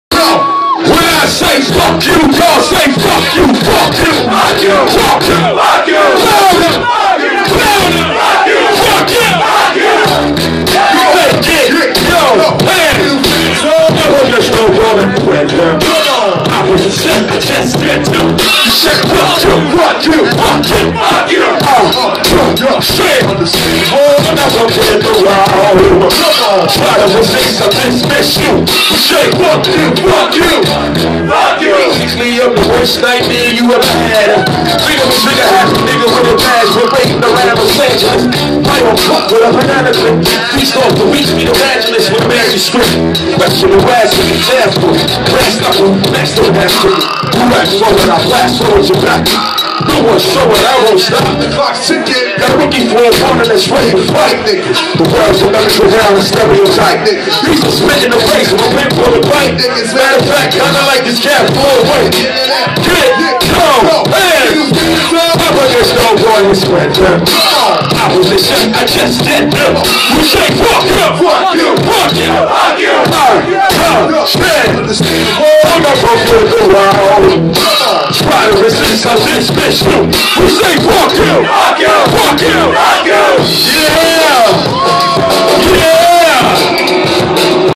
Pharoahe Monch & Dj Boogie Blind 4/14 @ Zoobar 22-12-13
Pharoahe Monch con Dj Boogie Blind (X-Ecutioners) direttamente da New York City a Roma allo Zoobar il 22 Dicembre 2013.

Nino
PALERMO CITY